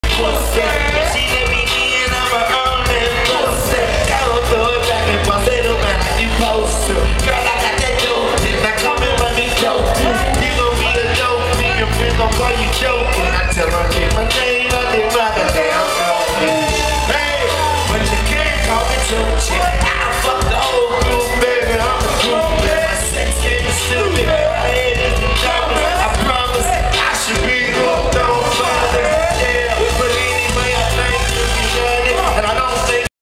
LIL WAYNE's little brothe, LIL TWIST rocks da Mic
LIL WAYNE concert in Hawaii. His little brother, "Lil Twist"